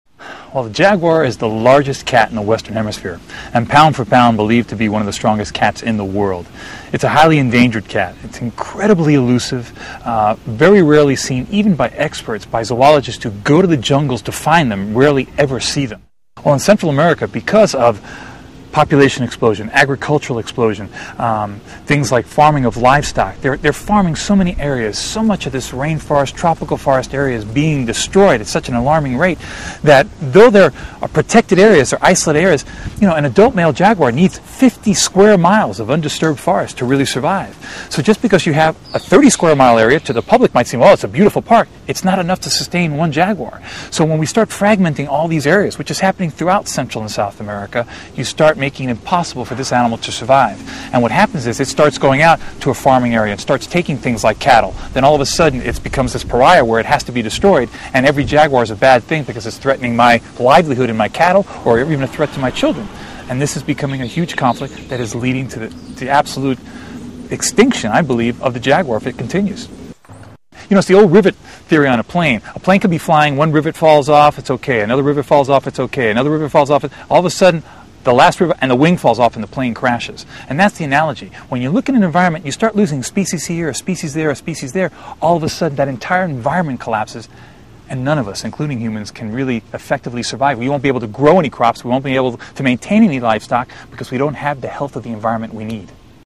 BELIZE on MSNBC: Jaguars In Danger: Belize Wildlife
Originally aired onMSNBC November 17th 2008, Ron Magill of the Miami Metrozoo comments on the Jaguar population in Central America. The Jaguar is the largest cat in the Western Hemisphere. Belize has probably the only Jaguar reserve in the world.